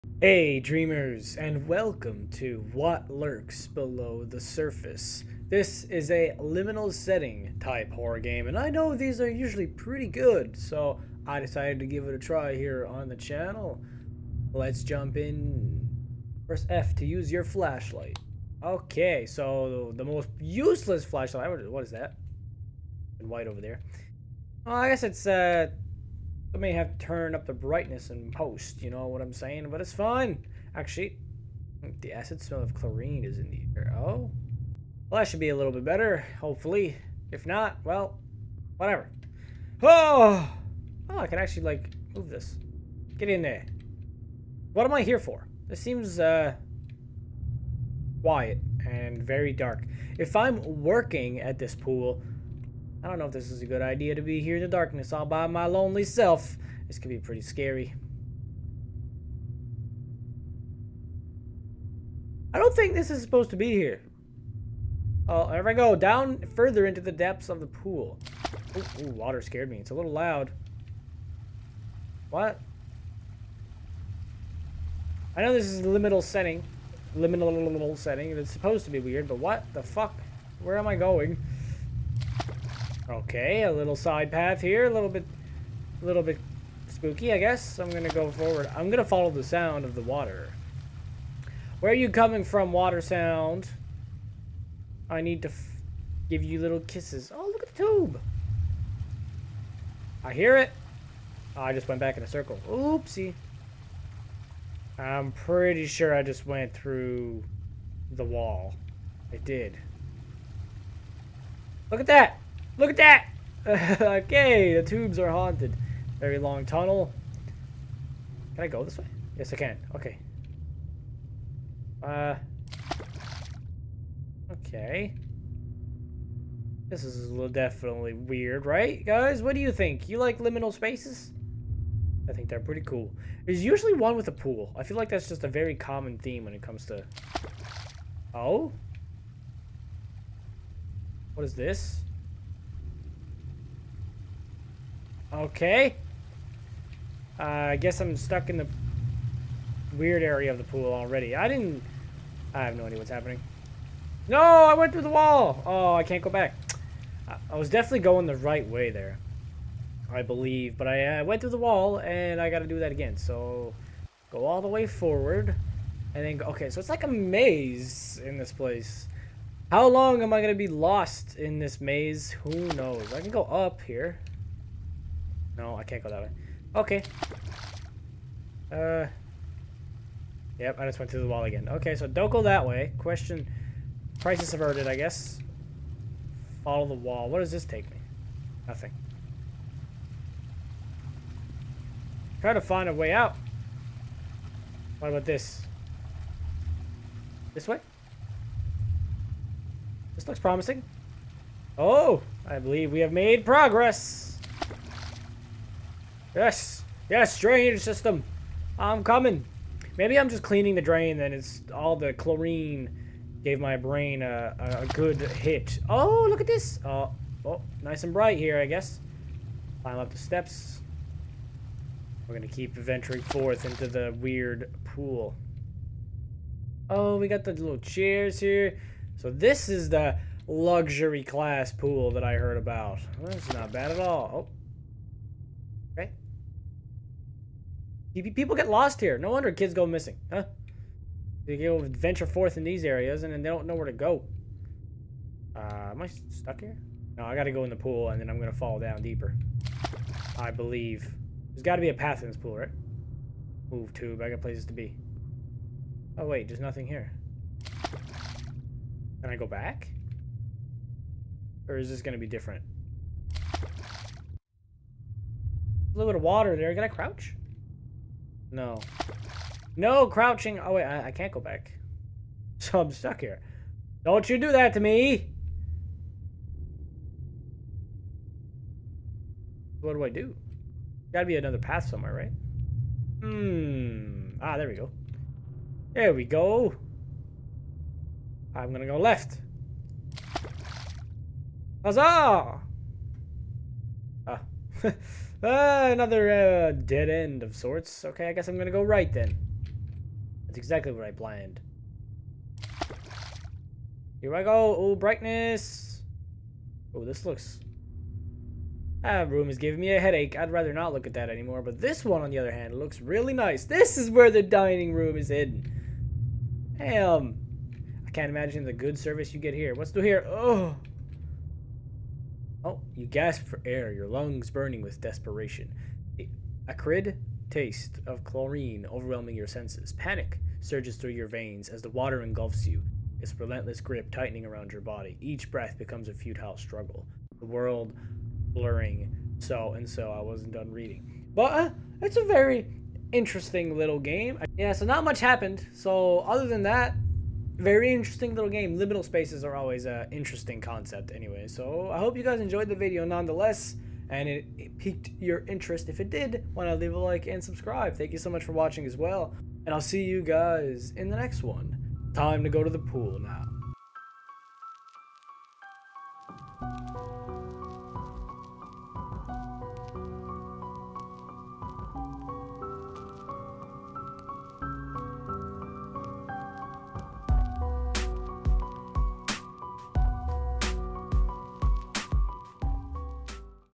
TRY NOT TO GET LOST CHALLENGE (IMPOSSIBLE) - What Lurks Below The Surface
A customer has been sucked down the drainage system. The manager wants you to have a look at it. I swear, one more of these and the whole (shabby) place might need to shut down for good!